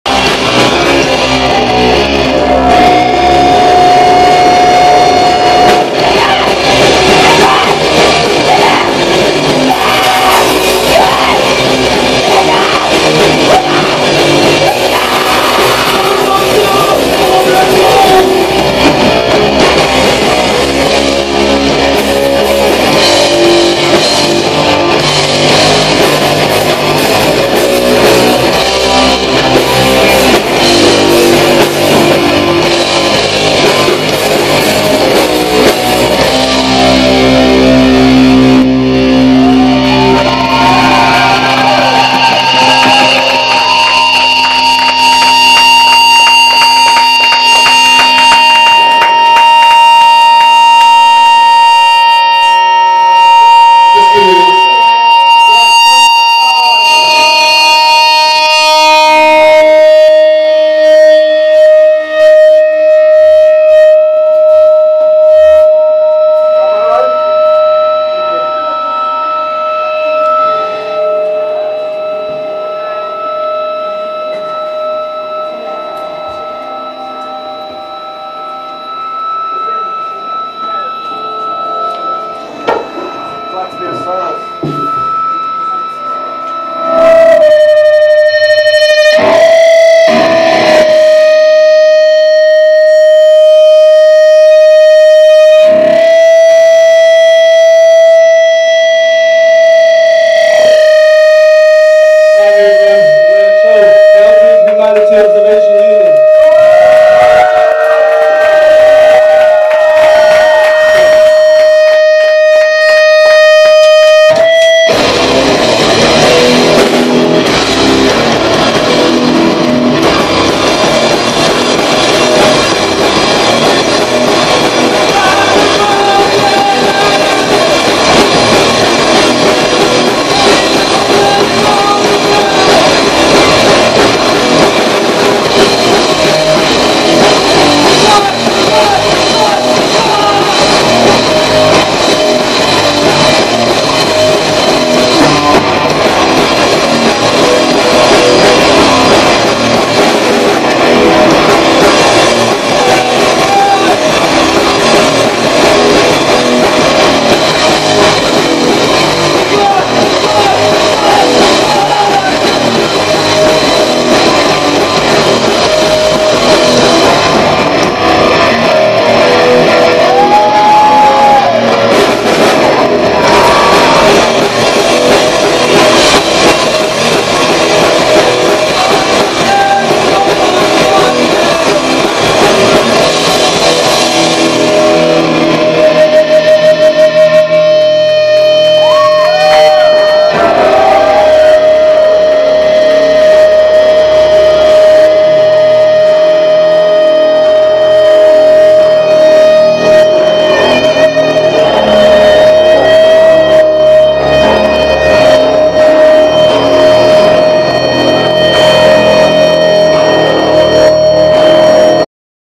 C.H.O.U. LIVE IN EDINDBURGH
some c.h.o.u. live in bannermans bar,edinburgh 10/9/24